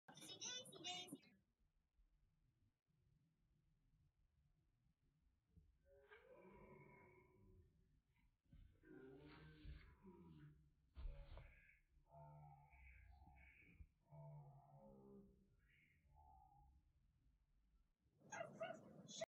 Cute bunny jumping in SLOW MOTION!!!
Watch this cute bunny hop through life